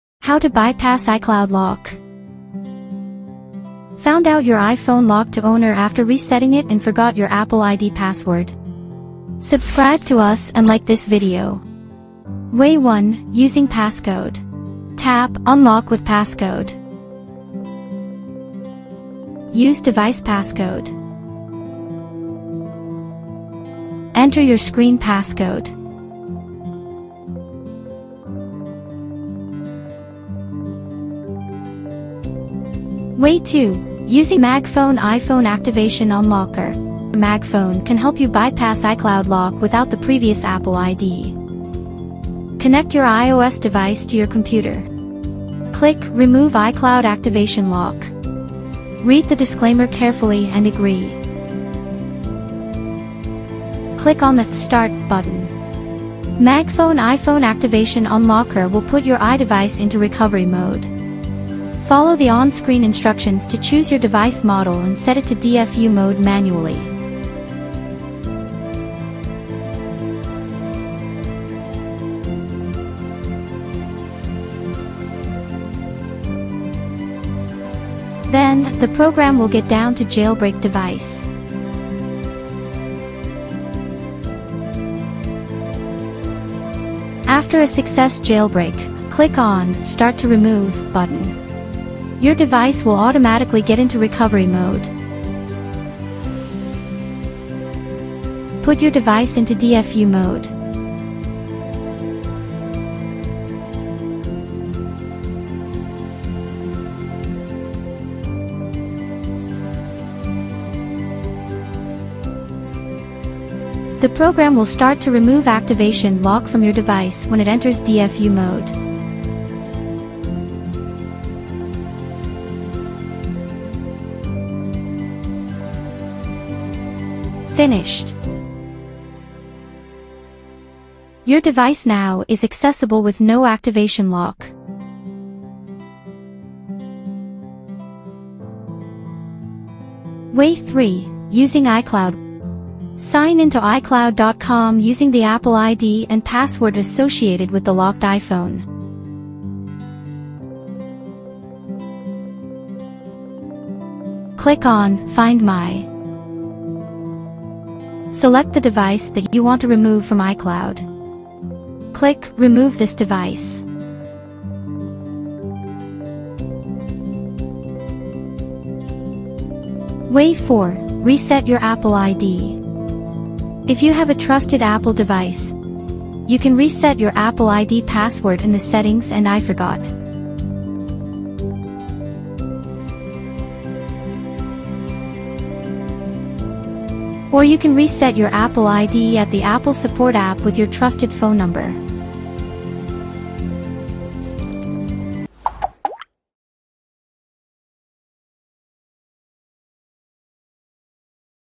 How to Bypass iCloud Lock without Password | MagFone
🔗Use MagFone iPhone Activation Unlocker to bypass iCloud lock without the previous Apple ID

What to do when you found out your iPhone locked to owner? If you’ve reset your iPhone and now it’s locked to the previous owner, and you’ve forgotten your Apple ID password, don’t worry! This video is here to help. We’ll guide you through step-by-step instructions on how to unlock your iPhone and regain access. Whether you’ve purchased a second-hand iPhone or accidentally locked your own, these tips and tricks will save the day. Learn how to bypass the iCloud activation lock, recover your Apple ID password, and use professional unlocking tools. Avoid common mistakes and ensure your iPhone is back in your hands quickly and securely. Let’s get your iPhone unlocked today!

Method Covered:
0:00 Introduction
0:12 Way 1: Using Passcode
0:35 Way 2: Using MagFone iPhone Activation Unlocker
2:21 Way 3: Using iCloud [Apple ID Required]
2:51 Way 4: Reset Your Apple ID